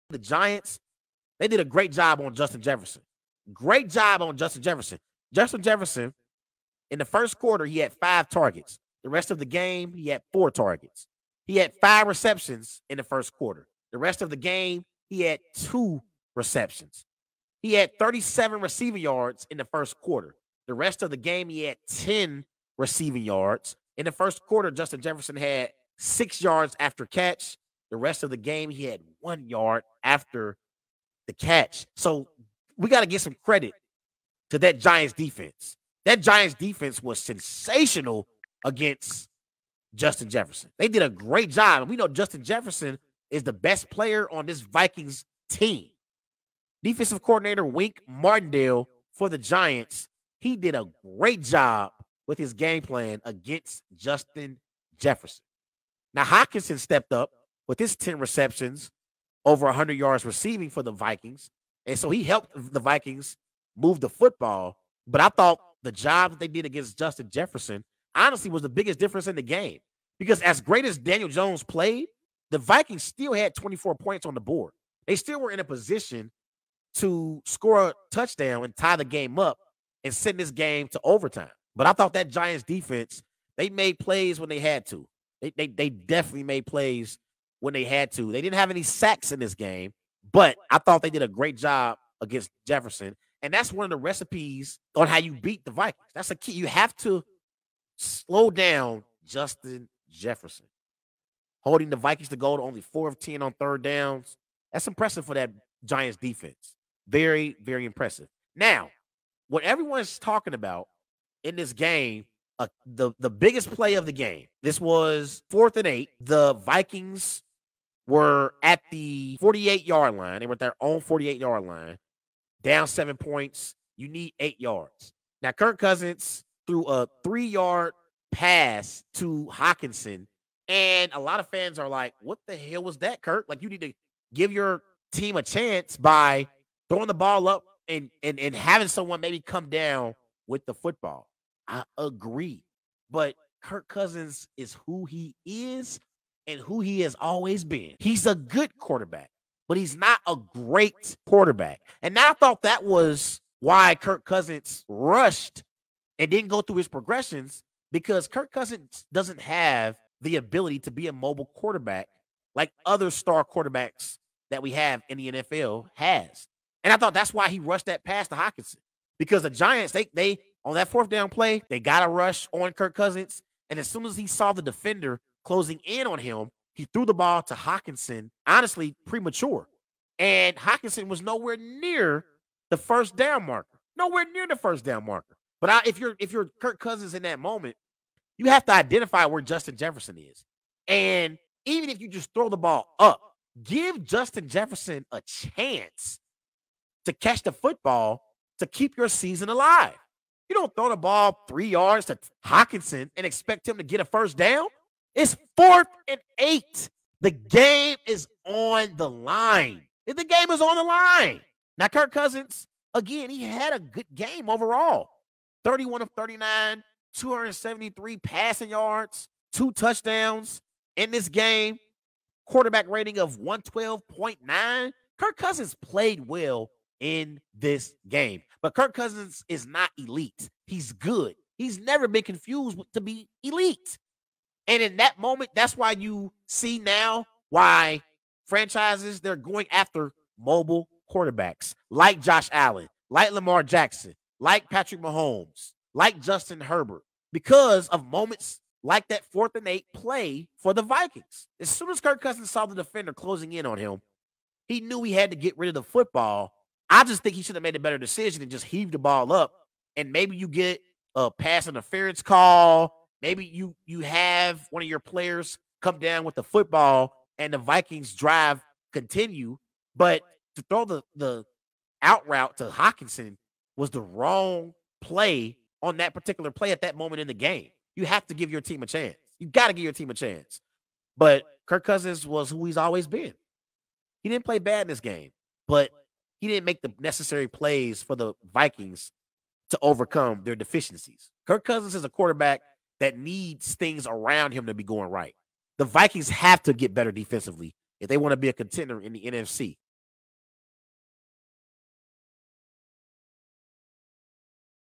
Kirk Cousins proving why teams draft more mobile quarterbacks?
Did the Giants do a tremendous job at containing Justin Jefferson in their playoff win against the Vikings? Has Kirk Cousins never been considered an elite quarterback or Top 10 quarterback? Did the Vikings' final play of the game where Kirk Cousins threw it short of the first down line show why teams draft more mobile quarterbacks in today's game?